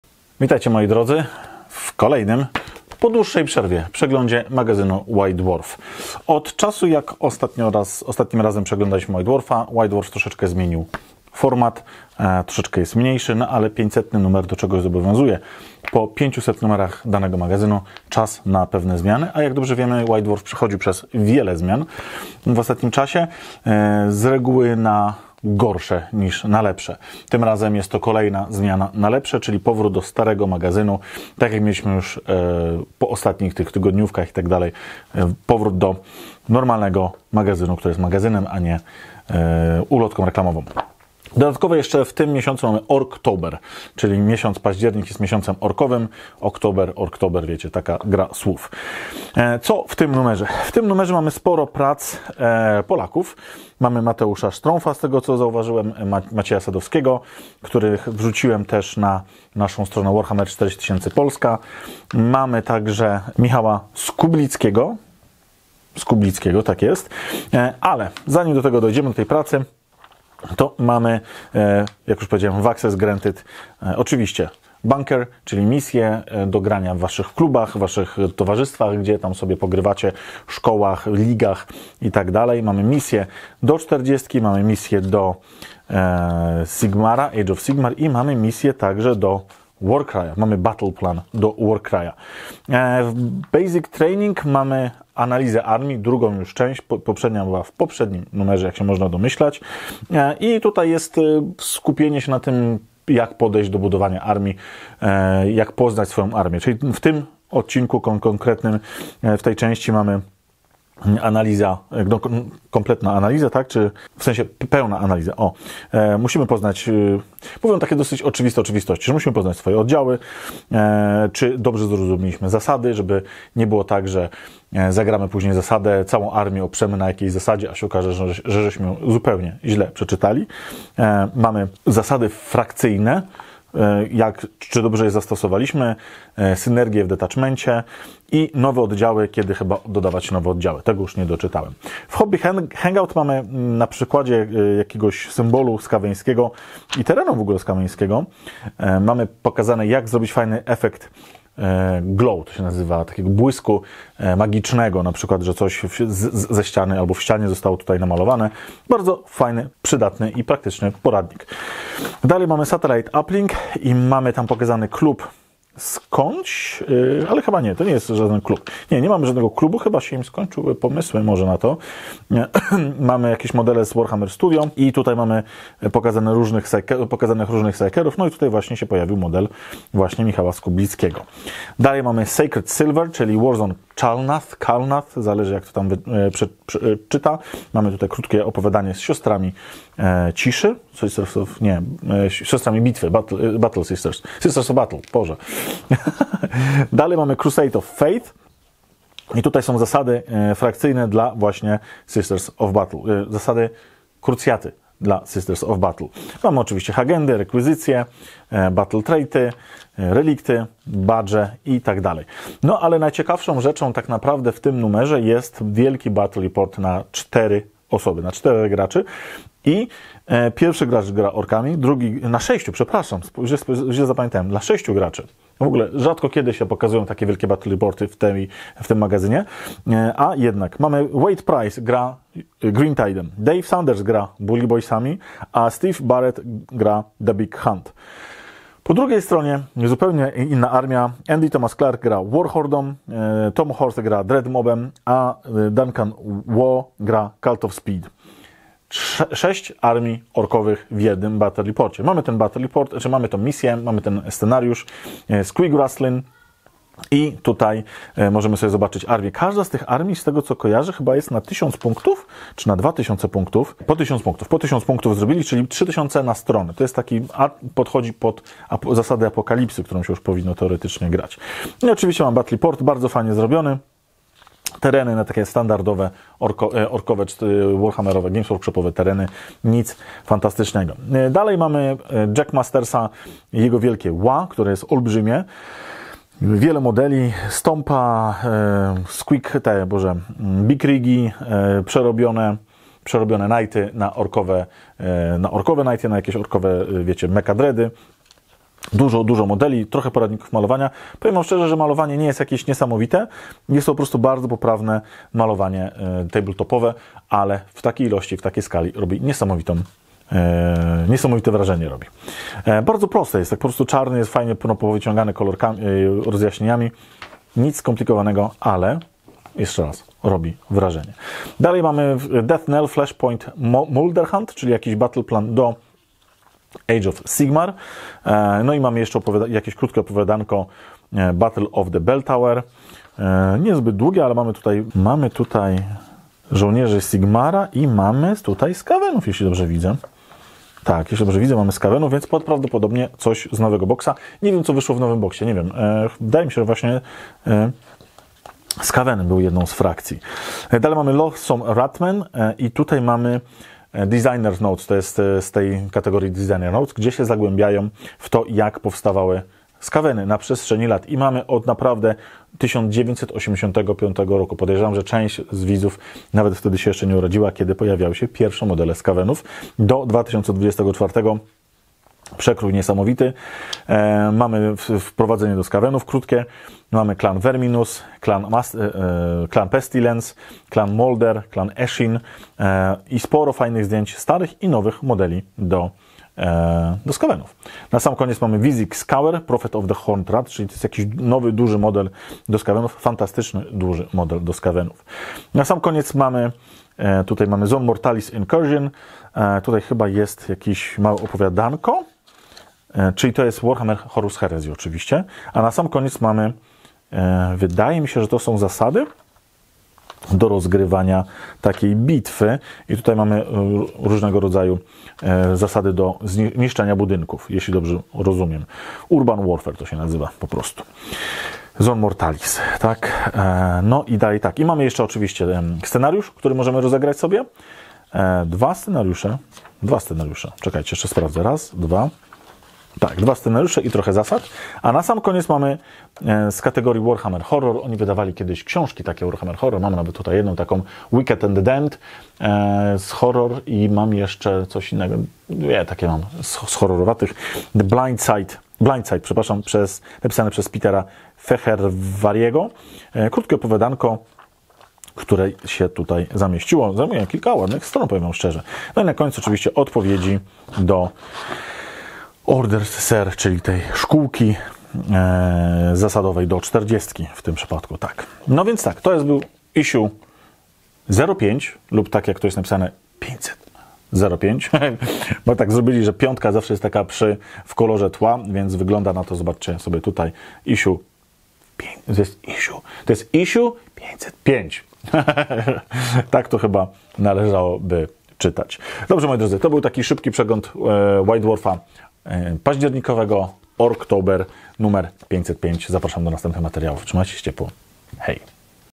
Przegląd zawartości magazynu White Dwarf 505
W tym odcinku przeglądam najnowszy numer magazynt White Dwarf 505 od firmy Games Workshop. W tym numerze materiały do systemów Warhammer 40k, Age of Sigmar, WarCry oraz Warhammer Horus Heresy.

Zapraszam!